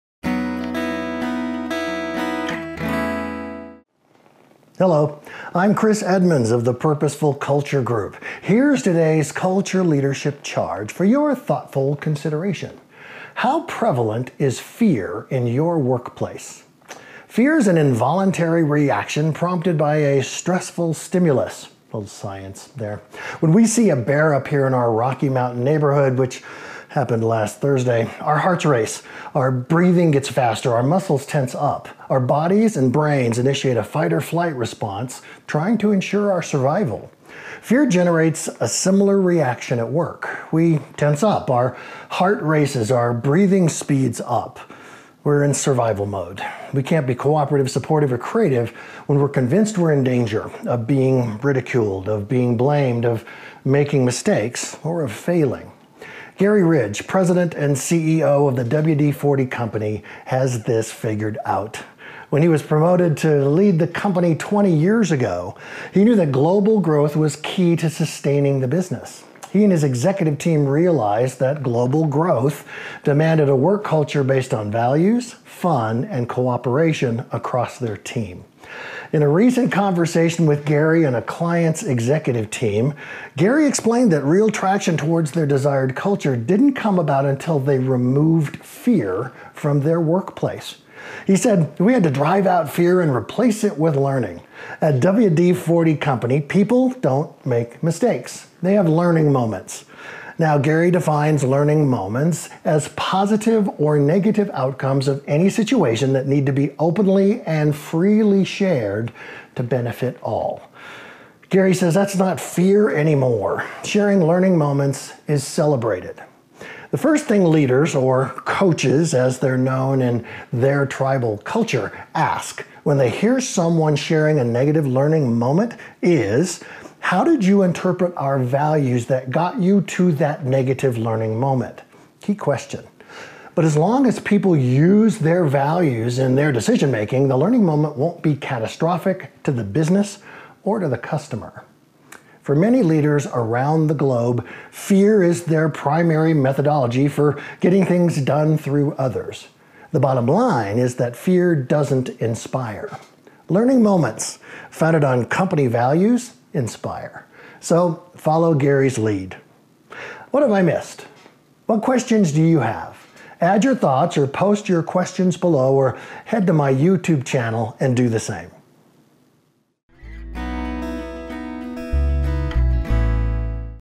Culture Leadership Charge, Episode 45 - Drive Out Fear
Here is episode forty-five of S. Chris Edmonds' Culture Leadership Charge video series. In these concise videos, Chris presents the best practices for creating and maintaining a purposeful, positive, productive culture - at work, at home, and in your community. Today's focus: Having a servant purpose, values and behaviors, strategies, and goals - alone - won't change your culture. You must drive out fear. Chris shares insights from conversation with Garry Ridge, President and CEO of WD-40 Company.